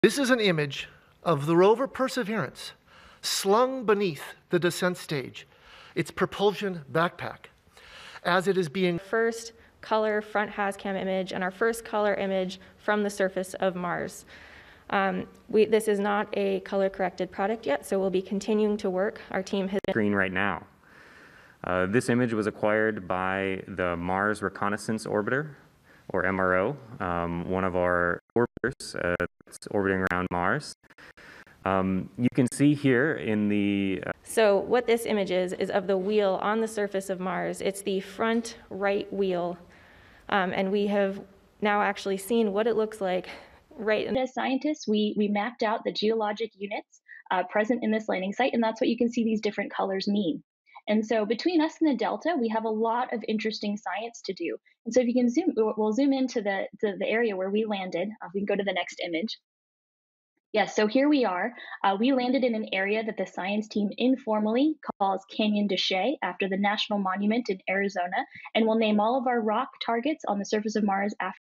NASA Rover Sends Back Color Pictures of Mars
Feb.22 -- NASA's Perseverance rover sent back the first color pictures from the surface of Mars. NASA equipped the spacecraft with a record 25 cameras and two mikes, many of which were turned on during Thursday's descent. (Source: NASA)